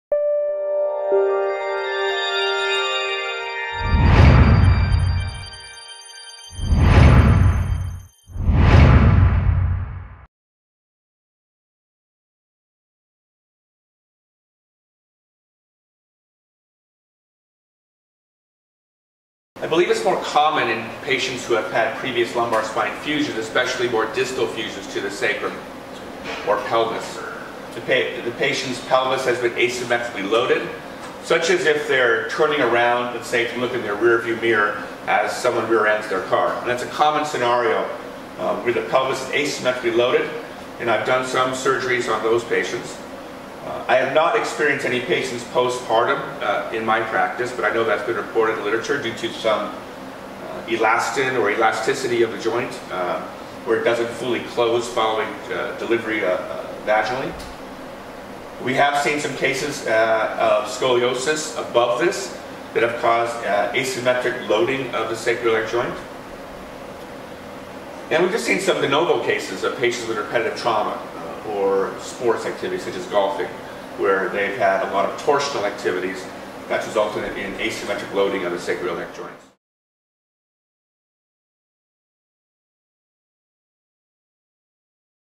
Causes of SI Joint Pain
The sacroiliac (SI) joint connects the sacrum, which is the lowest bony segment of the spine, with the lower limbs on each side. The SI joint transmits most of the weight loads from the upper body to the lower limbs, acting as a shock absorber, relieving forces on the spine. The SI joint is reinforced by strong ligaments, limiting motion in the joint.